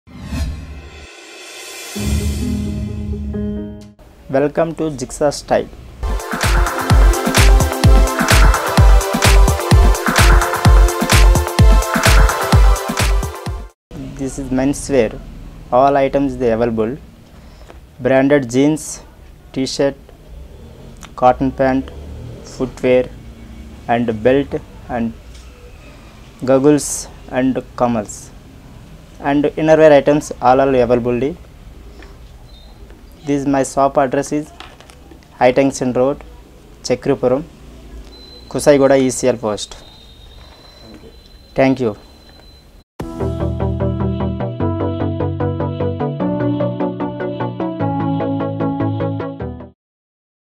Being Stylish Is My Hobby |Exclusive mens showroom @ jigsaw style| Kushaiguda | ECIL Post |zoneadds
Men in Blue Jeans is loved by every gentlemen. Jigsaw - Style menswear is available with casual shirts and jeans pant. Discounted prices so grab the offer. Shop located at Kushaiguda, ECIL, Hyderabad.